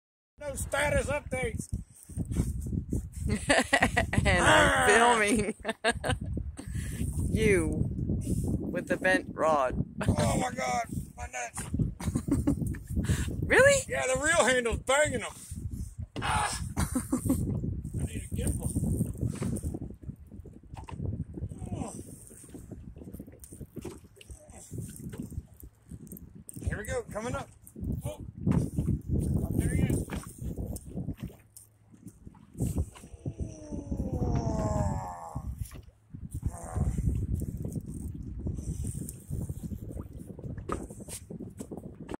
Jack Crevalle Status Update
Caution: Adult Language is used... Here is a great example of a down and dirty approach to fighting big fish like Jack Crevalle. The angle of the rod really determines the length of fight. When these jacks get under the boat and lay on their sides they will swim in circles in order to rest and conserve their energy. The only way to get their head up is to crank down the rod at a low angle and use the reel to winch them in slowly, one inch at a time. Just pray they dont take any great lengths of it over and over!